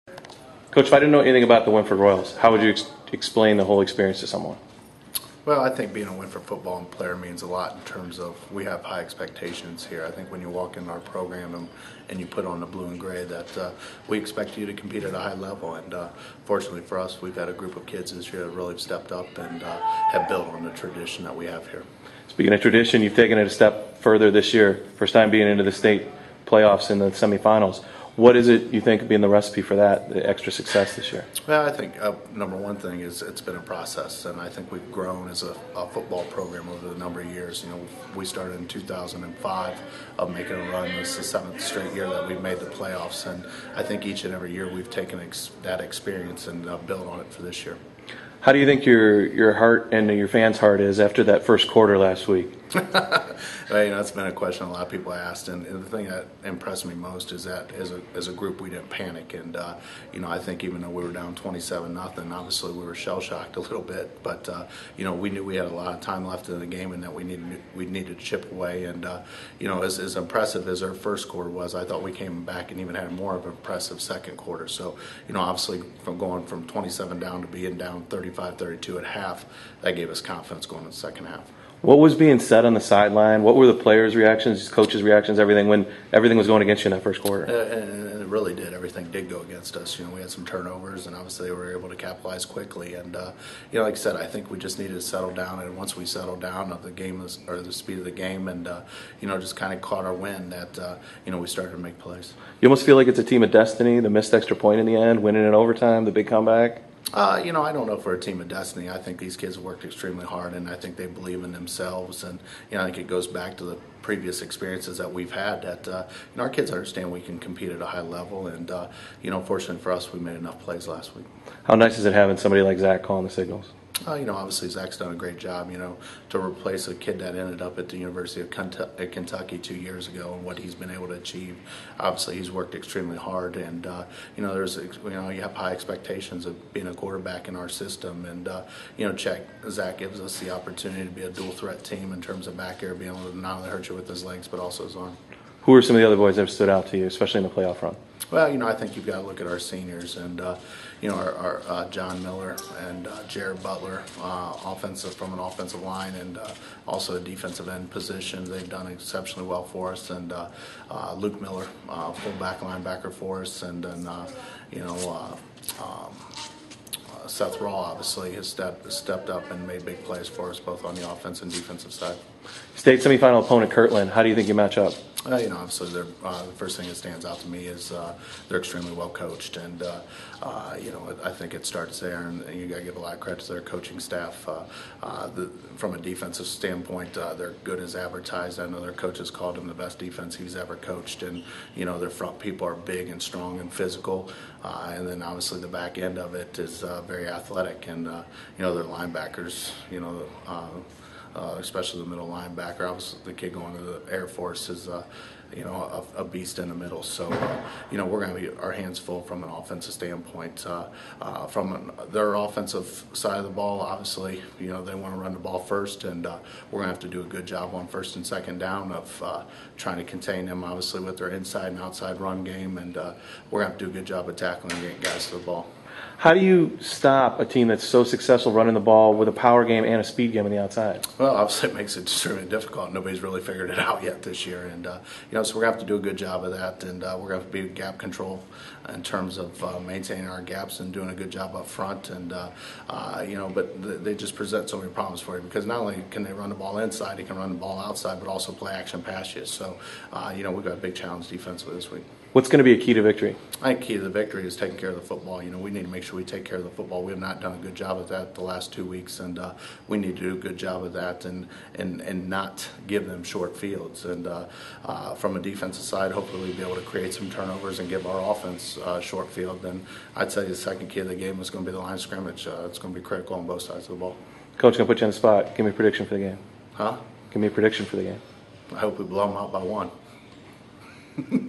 Wynford Royals pregame
Interview w/HC Moyer for OhioSportsNet's broadcast Kirtland vs Wynford
D5 state semifinal 11-25-11